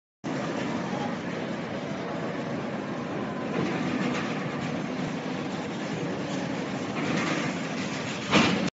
Aluzinc steel coils that have finished packaging
Aluzinc steel coils that have finished packaging-Shanghai Zhengshang Industrial Co., Ltd.
There are thousands of tons of produced galvanized steel coils in our warehouse, and they are packed into 3-20 tons per coil. These coils will use fumigated wooden pallets, waterproof paper, and iron sheet packaging, which can effectively ensure the safety during transportation, and also ensure that the aluminum-zinc steel coils are stored for a long time without rusting.